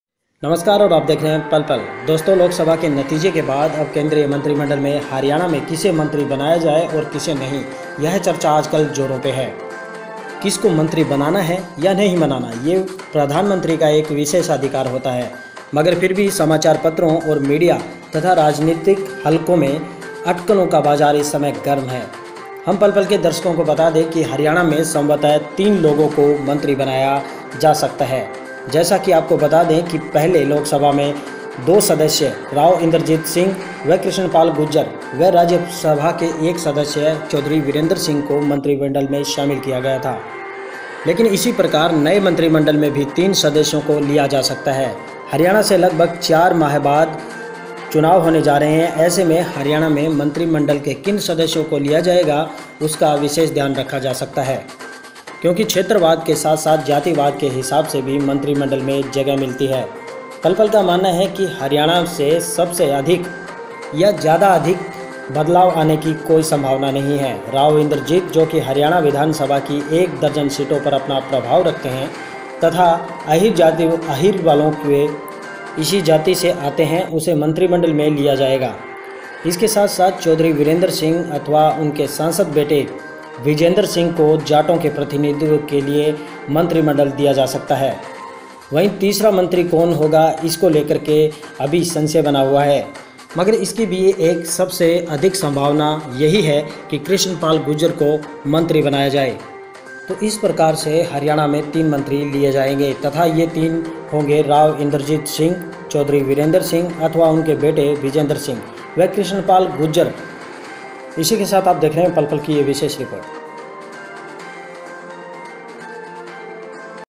जाने हरियाणा में किसकी निकलेगी लाटरी कौन बनेगा मंत्री
लोकसभा के नतीजों के बाद अब केंद्रीय मंत्रिमंडल मैं हरियाणा से किसे मंत्री बनाया जाएगा इस पर चर्चा जोरों पर है किसी को मंत्री बनाना अथवा ना बनाना प्रधानमंत्री का विशेषाधिकार होता है मगर फिर भी समाचार पत्रों मीडिया राजनीतिक हलकों में अटकलों का बाजार गर्म है हम पल पल के दर्शकों को बता रहे हैं कि हरियाणा में संभवत तीन लोगों को मंत्री बनाया जा सकता है जैसा कि आपको बता दें पहले भी लोकसभा के 2 सदस्यों Rao Inderjit Singh Krishan pal Gurjar व राज्यसभा से एक सदस्य चौधरी बिरेंदर सिंह को मंत्रिमंडल में शामिल किया गया था इसी प्रकार नए मंत्रिमंडल में भी तीन सदस्यों को लिया जा सकता है हरियाणा में लगभग 4 माह बाद चुनाव होने जा रहे हैं ऐसे में हरियाणा में मंत्रिमंडल में किन सदस्यों को लिया जाएगा उसका विशेष ध्यान रखा जा सकता है क्षेत्रवाद के साथ साथ जातिवाद के हिसाब से भी मंत्रिमंडल में जगह मिलती है पल पल का मानना है कि हरियाणा से अधिक बदलाव आने की कोई संभावना नहीं है राव इंदरजीत जो हरियाणा विधानसभा की 1 दर्जन सीटों पर अपना प्रभाव रखते हैं तथा आहिर वालों से आते हैं उन्हें मंत्रिमंडल में लिया जाएगा उनके साथ साथ चौधरी बिरेंदर सिंह अथवा उनके सांसद बेटे बृजेंद्र सिंह को जाटों को प्रतिनिधित्व देने के लिए मंत्रिमंडल में लिया जा सकता है तीसरा मंत्री कौन होगा इसको लेकर अवश्य संशय हो सकता है मगर इसकी भी सबसे अधिक संभावना यही है की कृष्ण पाल गुज्जर को ही मंत्री बनाया जाए तो इस प्रकार हम आपको बता रहे हैं कि हरियाणा से तीन मंत्री लिए जाएंगे तथा वे तीन होंगे राव इंदरजीत सिंह चौधरी बिरेंदर सिंह अथवा उनके बेटे बृजेंद्र सिंह व कृष्ण पाल गुज्जर तो आप देखते रहिए पल-पल की विशेष रिपोर्ट